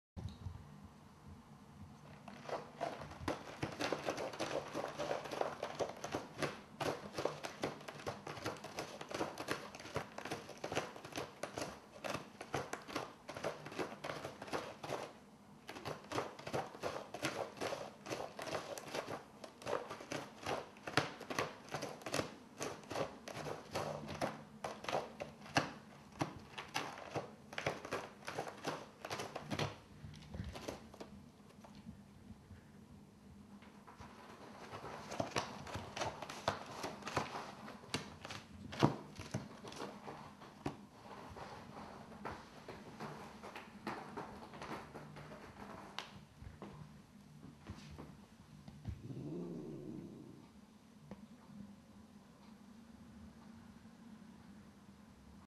These boots were made for scratchin'
Rocky loves breaking in a brand new scratch box while Gigi is stuck with the old one. If you turn up the volume, you'll hear Gigi growl near the end. Video responses welcome:)